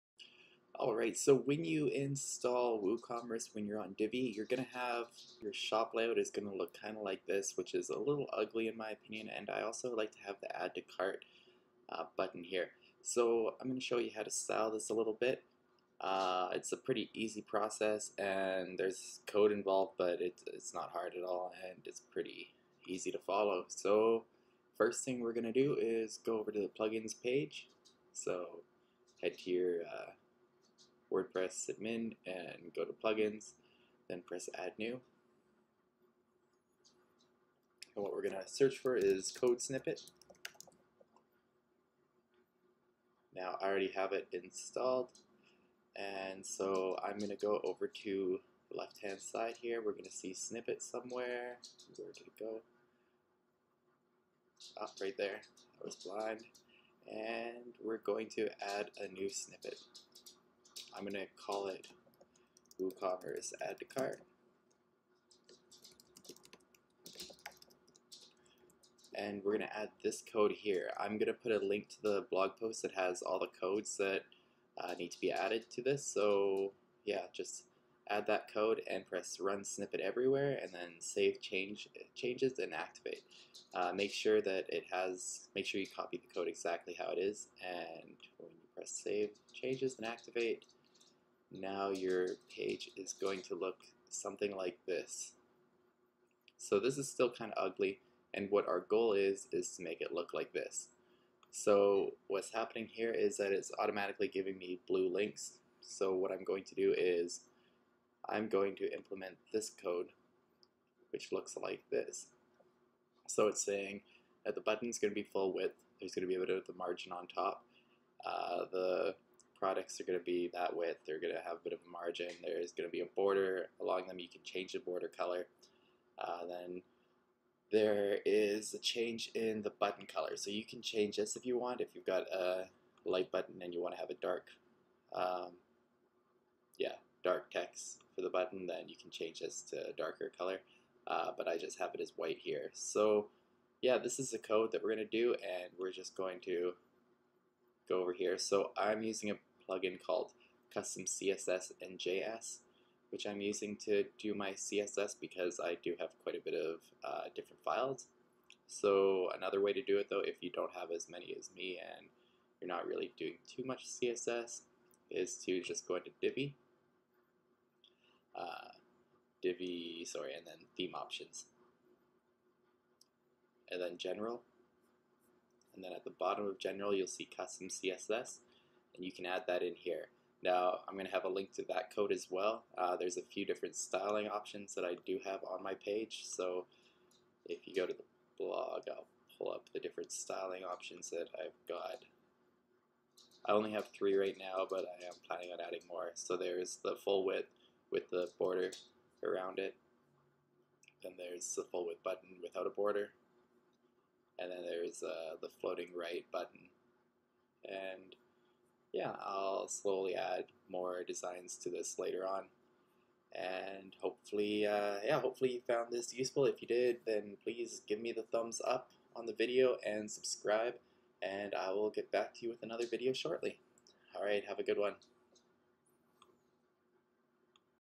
Customize Divi with WooCommerce | Add-To-Cart Button | CSS Styling
Here are some customization tips when you're using Divi with Woocommerce.